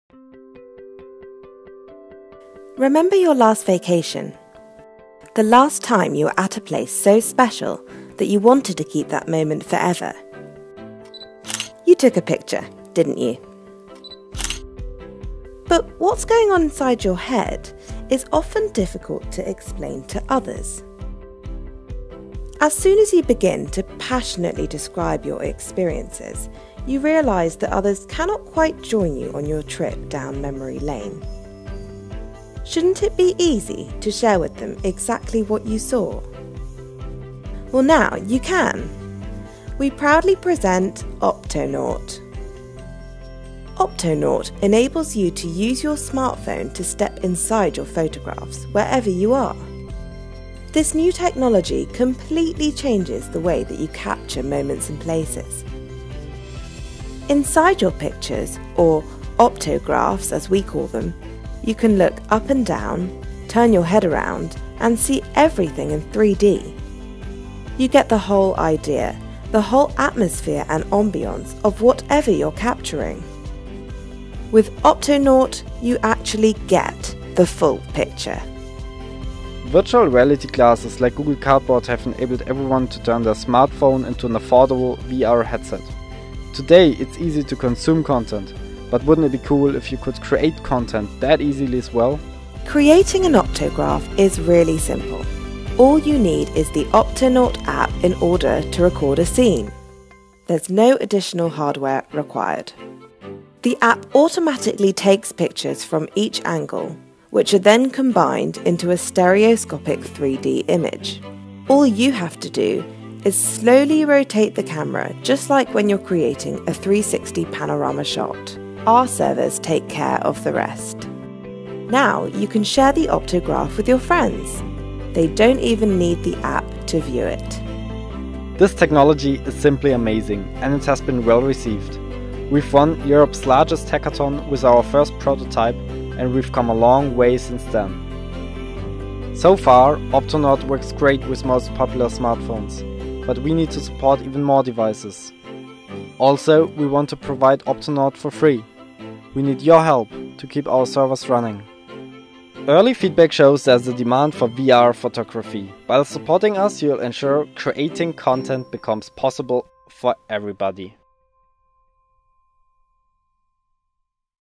Optonaut - Virtual Reality Photography
You can think of Optonaut as an Instagram for Virtual Reality.

Optonaut is an app that lets you capture and share places in 3D using your smartphone. Your friends can experience these places as if they were there themselves. All you need are Virtual Reality glasses like Google Cardboard. And the best part: for capturing, there is no additional device needed at all.